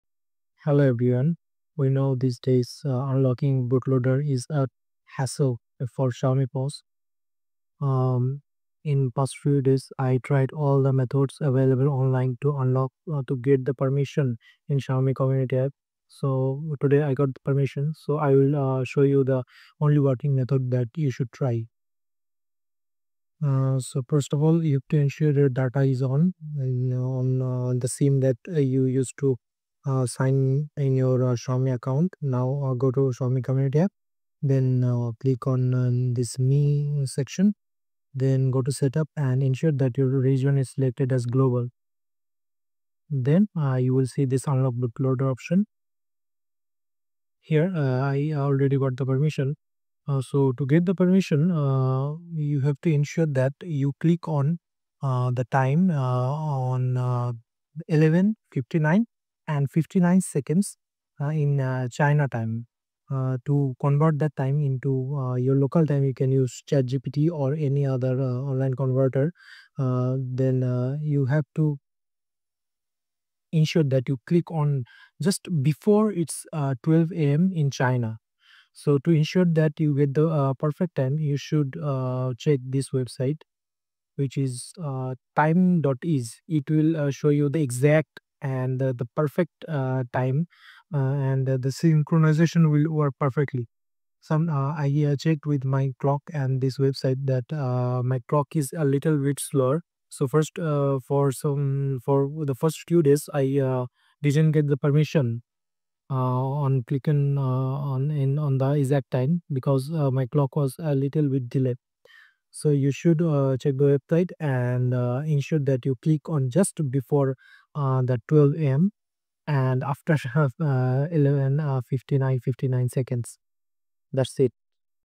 Xiaomi Bootloader Unlock Permission Bypass 100% Working Method | Hyper OS 2 | Xiaomi Community
In this video, I’m sharing how I managed to get the bootloader unlock permission on my Poco M6 Pro 5G (codename: sky) after HyperOS update. It kept saying "limit reached" every time I tried, but I finally figured out the perfect timing trick that actually worked!

No need for VPN, no extra software — just timing and patience. Ensure the requirements

🕒 I clicked right 500ms before 00:00:00 CST and it worked.
📱 Xiaomi Community app, latest version
🔐 Developer options enabled, Mi account linked
🧠 Timing is everything — watch the video for exact steps I followed

Hope this helps you unlock your device too. Let me know if it worked for you!